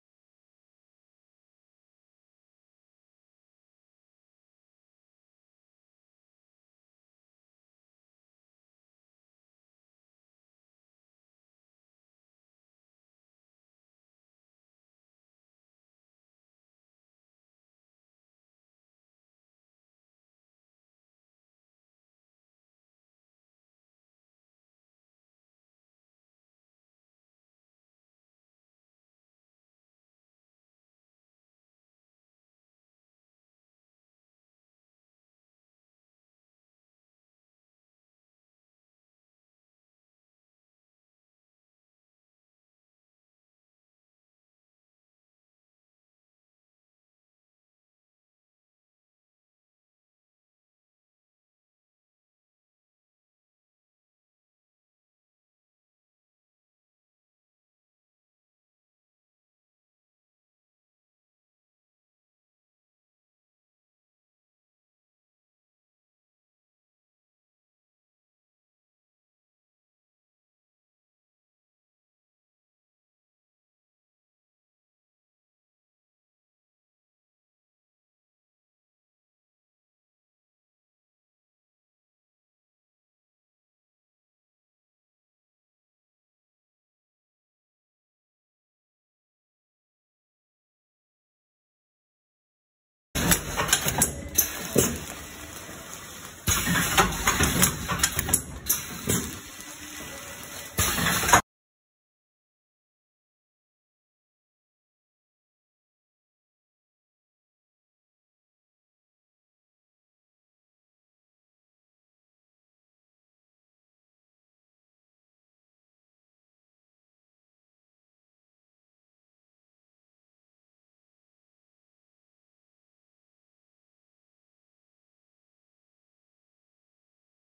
MYWAY - packaging machine for Stand up pouch bag
MYWAY e una confezionatrice pensata per realizzare sacchetti con fondo quadro, partendo da una bobina di film steso, il tutto con una sola macchina.
Questa macchina possiede molteplici optional come :
-dispositivo piega film
-doppia punta per fondo quadro
-punzone per foro maniglia
-punzone per forare il sacchetto
-zip per rendere il sacchetto richiudibile

Per maggiori informazioni contattaci :
Gami s.r.l.
Via Lago di Bolsena, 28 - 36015  Schio  (VI) Italy

(EN)
MYWAY is a packaging machine designed to make bags with a square bottom, starting from flat film.
This machine has multiple options such as:
-film folding device
-double tip for square bottom
-punch for handle hole
-punch to pierce the bag
-zip to make the bag resealable

Gami s.r.l.